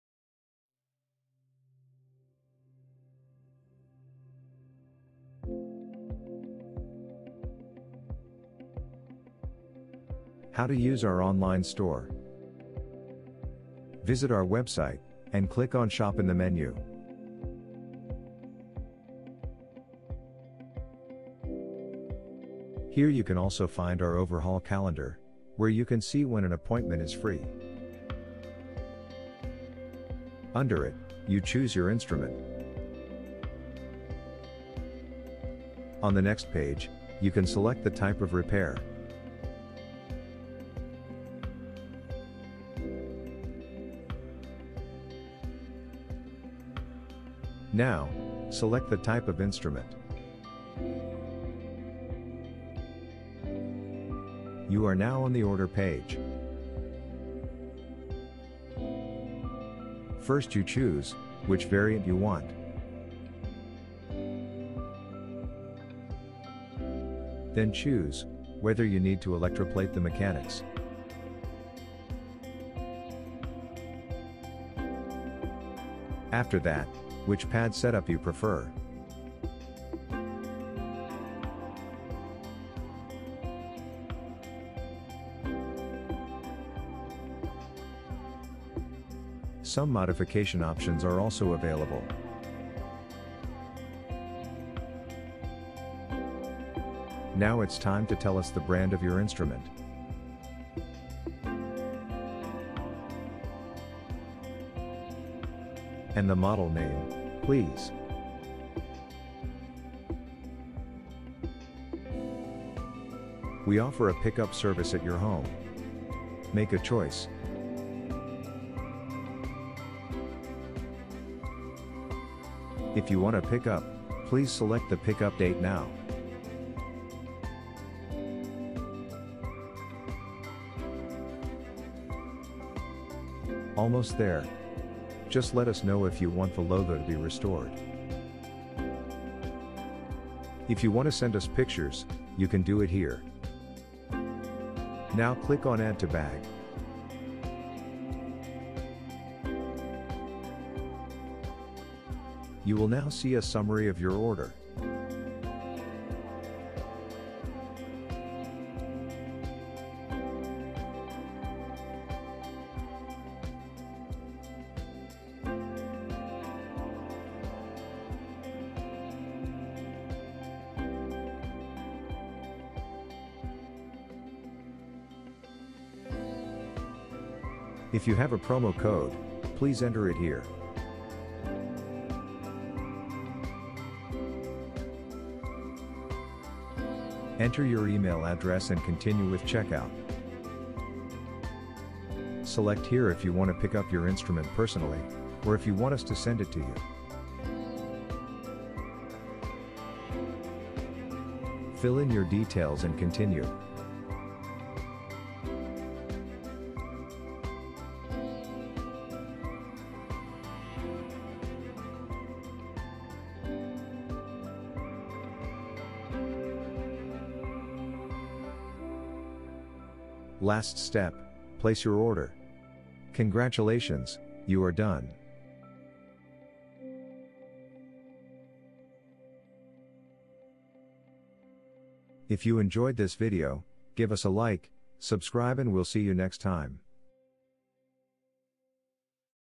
How to order a repair in our Webshop!
This video shows you how to order a repair in our webshop. All overhauls must be ordered via the webshop. We are happy to help you with this.